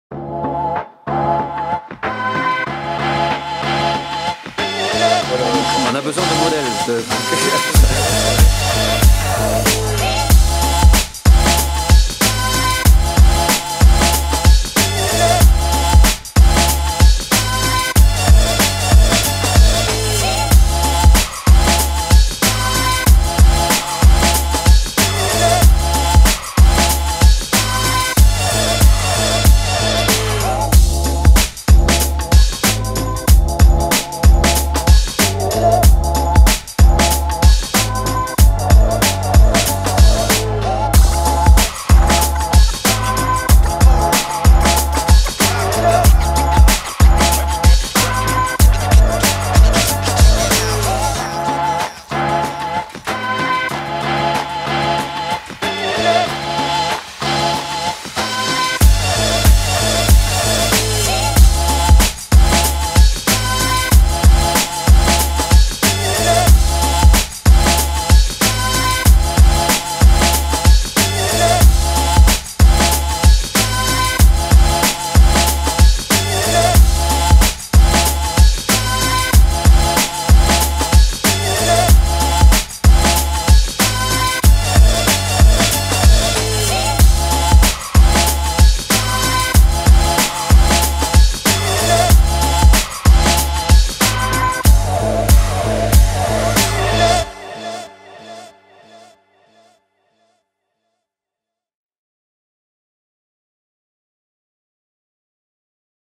Making Of Good Karma
Voici la vidéo backstage du shooting de l'Agence de communication Karma 2018